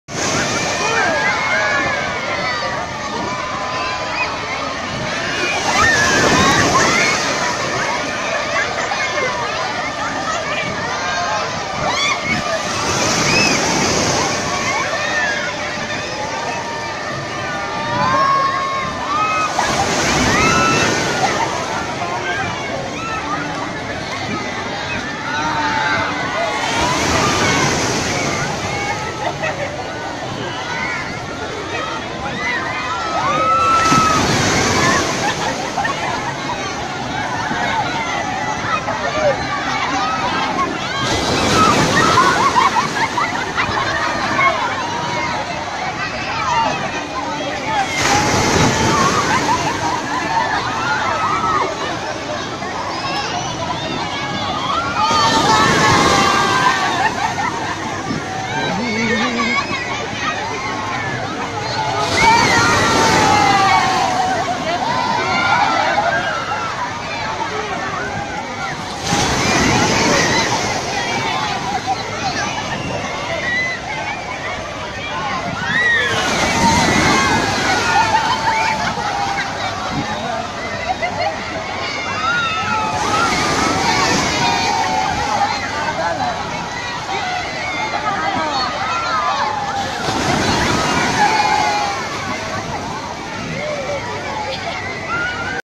Waterpark Kertosono
serunya ombak tsunami di waterpark kertosono.